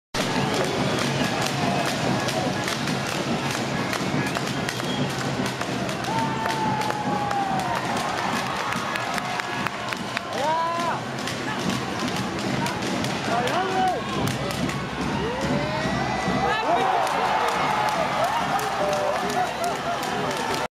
Rene Rougeau, enceste con engaño
Buena jugada de Rene Rougeau, en el juego definitivo contra Huracanes de Tampico el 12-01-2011